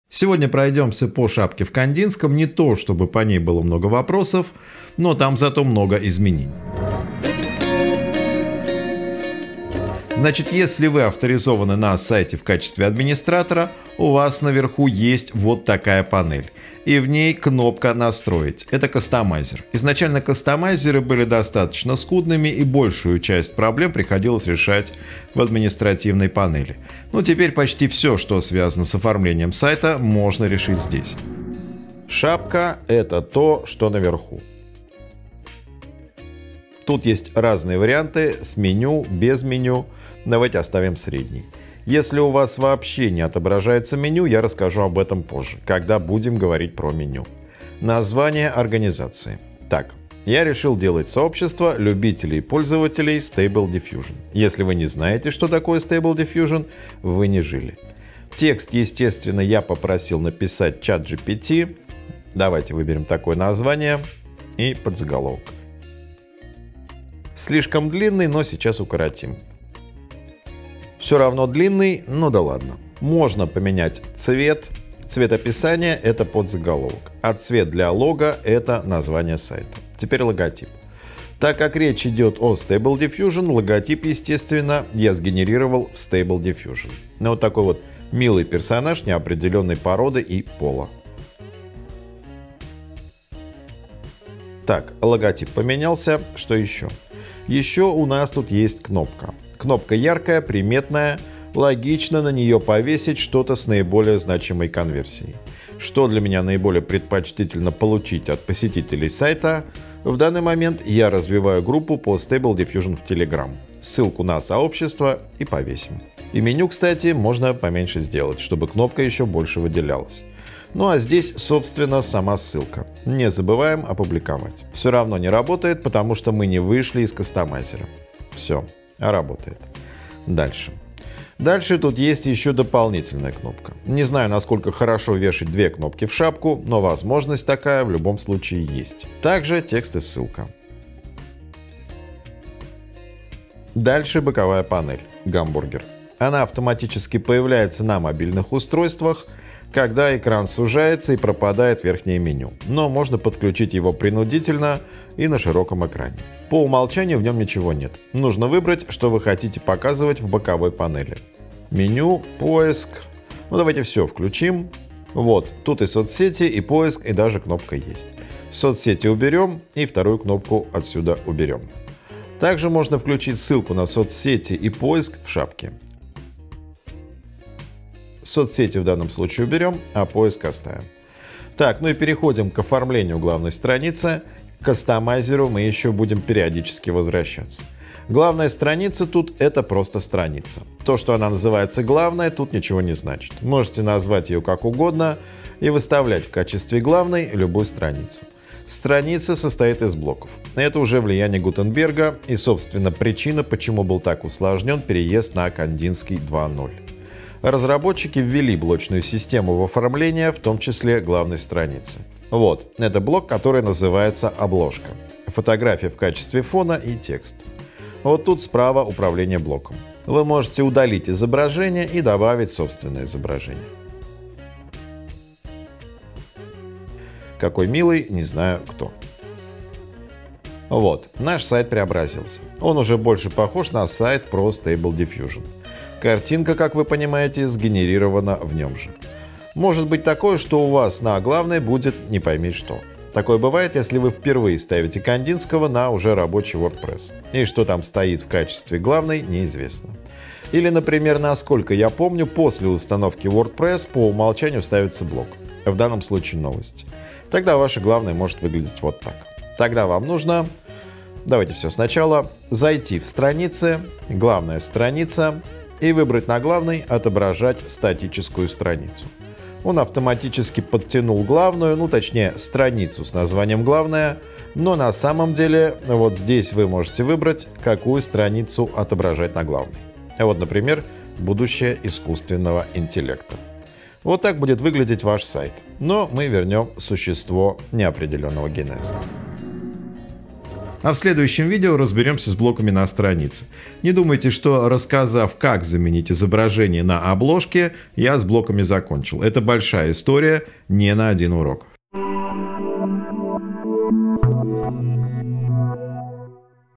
Кандинский 2.0 #2: оформление шапки, как выбрать главную страницу
Разработка Теплицы конструктор сайтов «Кандинский» полностью обновился больше года назад. Мы делали небольшой курс по «Кандинскому», когда он был в бета-версии, а теперь сделали полный курс по итогам года работы. В этом курсе мы постарались ответить на все вопросы, которые присылали по «Кандинскому» в службу поддержки.

«Кандинский» – это бесплатная тема для WordPress, разработанная Теплицей специально для НКО. Это коробочное решение и после установки «Кандинского» вы получаете полностью рабочий сайт со всеми необходимыми плагинами.

Пример выравнивания текста и округления углов изображения через дополнительные стили CSS:

.knd-block .section-title
{margin-left: auto;
margin-right: auto;
text-align: center;}

Body { --knd-image-border-radius:10px }